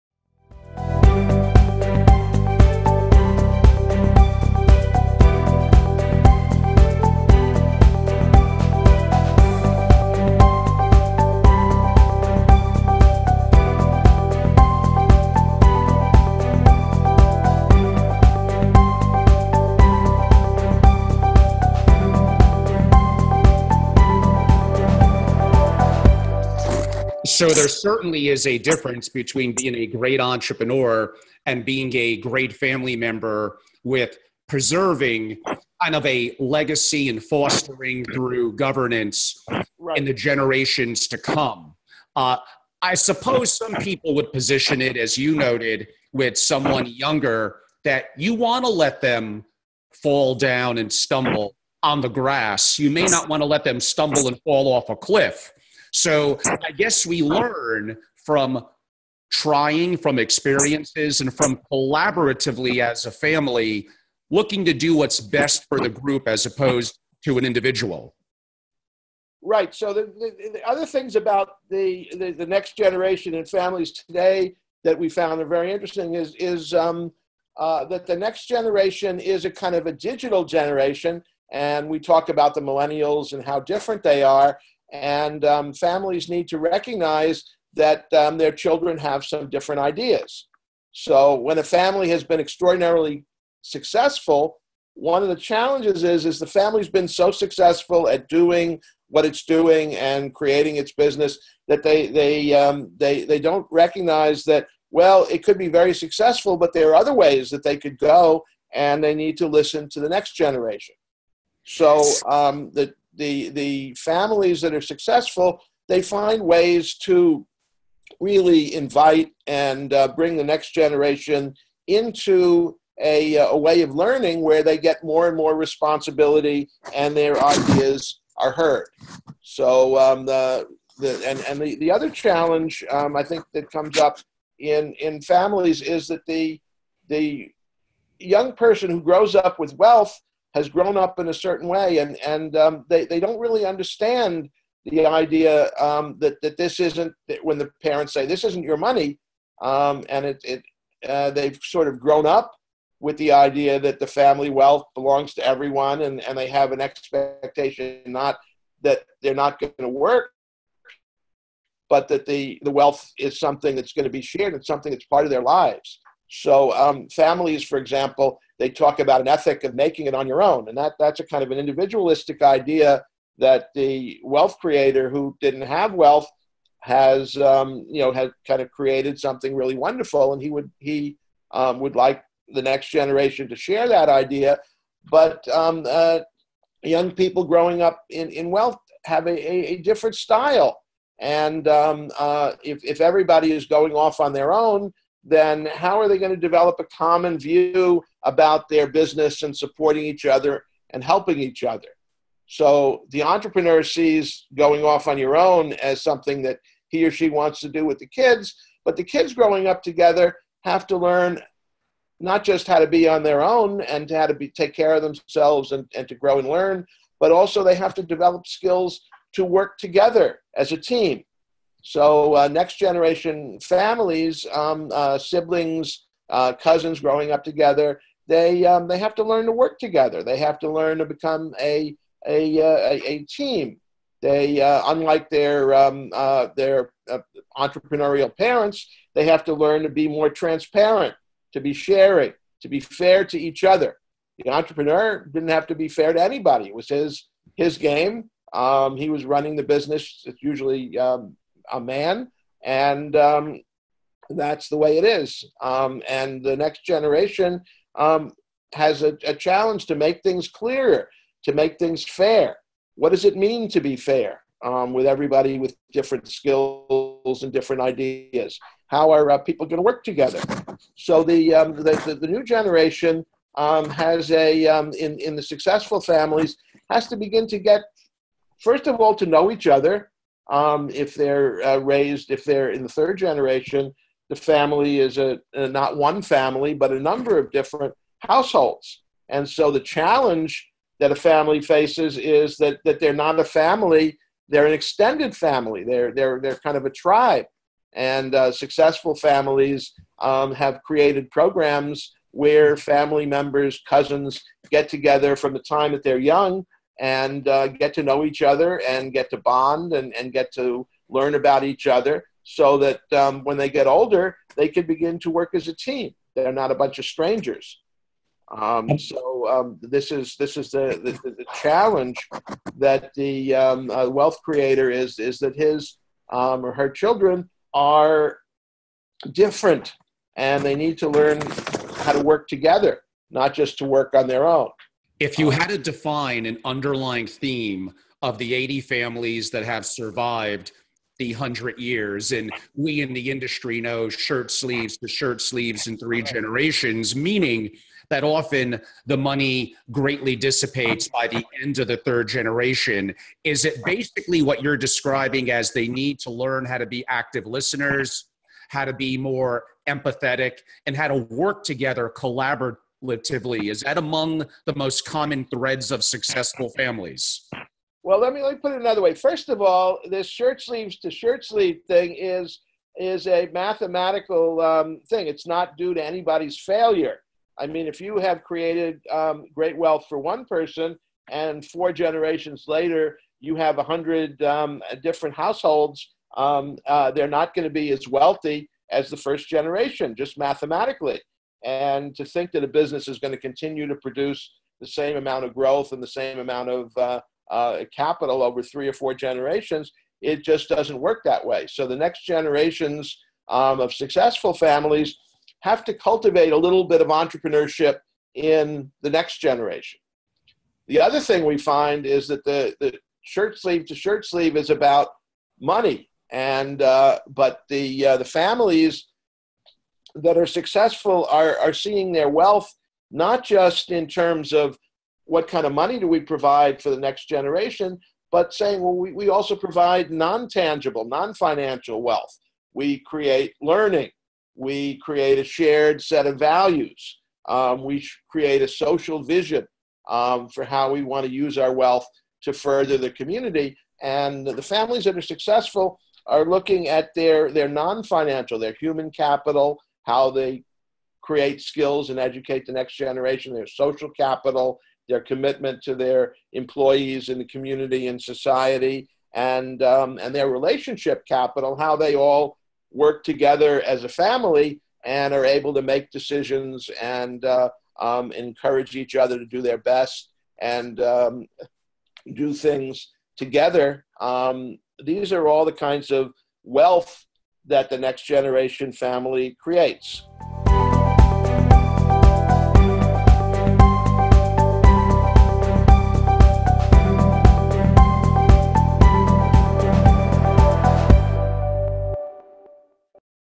Entrepreneur Parents vs Millenial Children
In this episode, Angelo discusses the upcoming trends emerging from the next generation of family members within the single-family office enterprise with special guest, Dennis Jaffe.

Want more on 'Next Generation Trends in the Family Enterprise'?